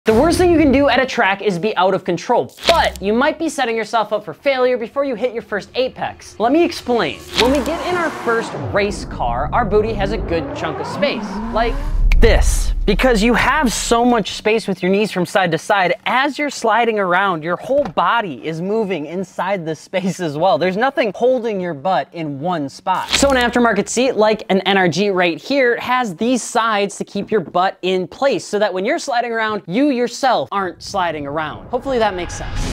Get An Aftermarket Seat Before THIS Happens 😳
An aftermarket seat has a TON of benefits. From holding you in place, to even making your interior look a bit better. Hear what Alex has to say on todays short!